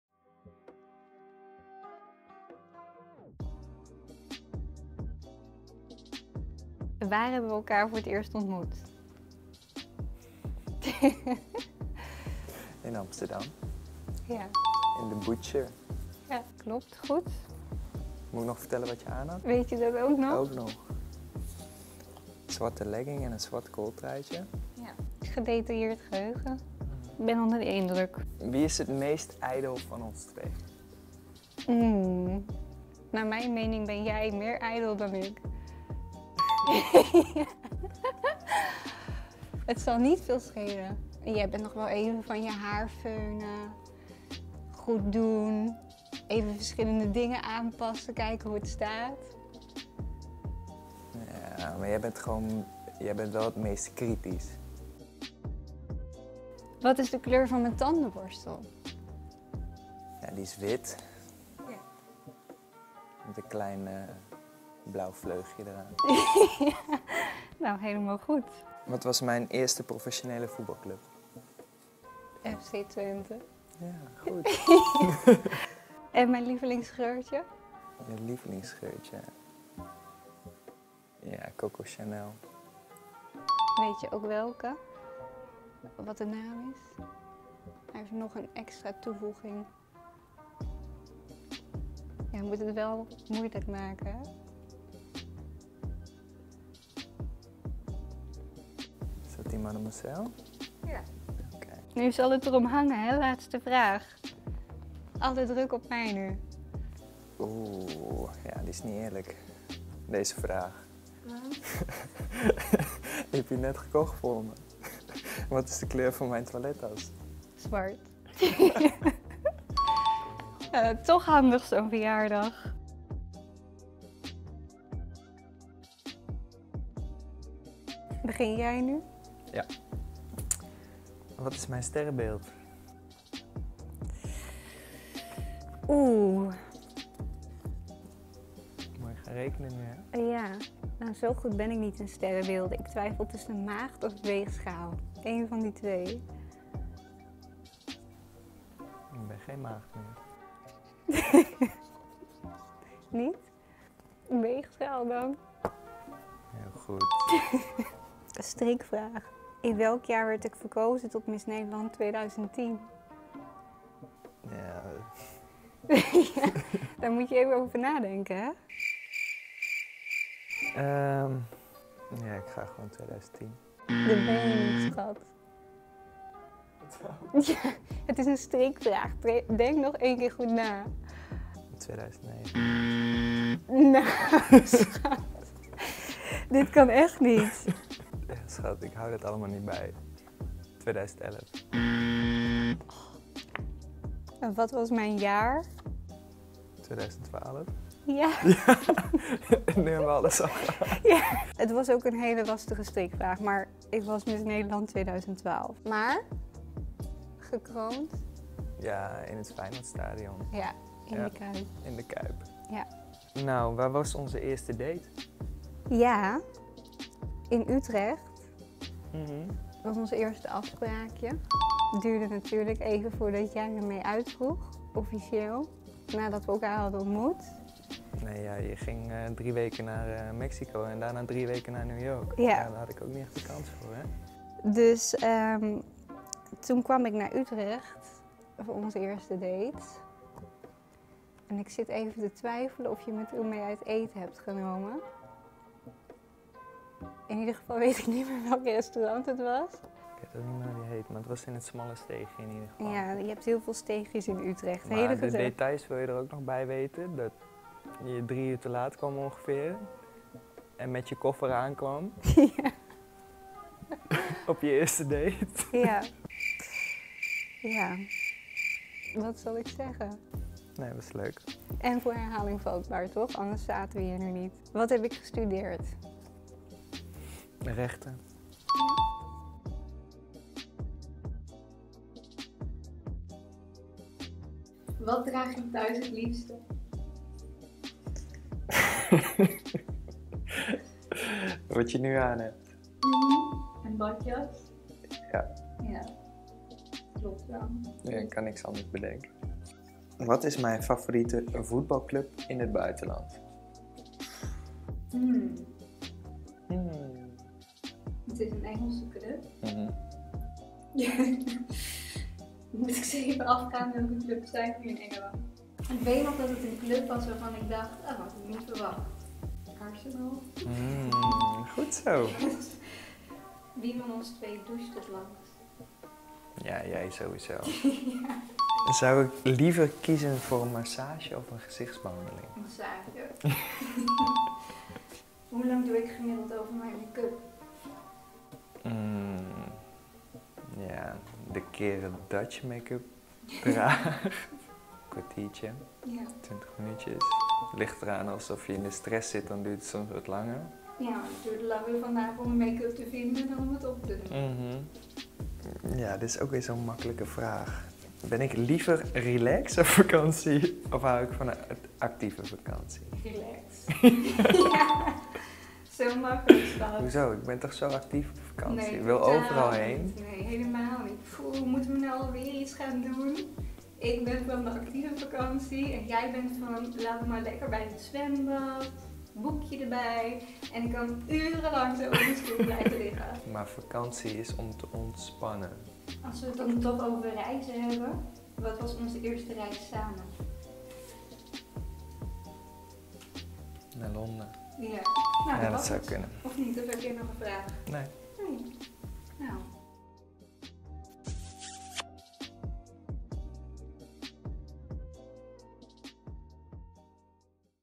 Switch Play - Nick Marsman & Nathalie den Dekker
In Switch Play Nick Marsman en Nathalie den Dekker ask each other some tough questions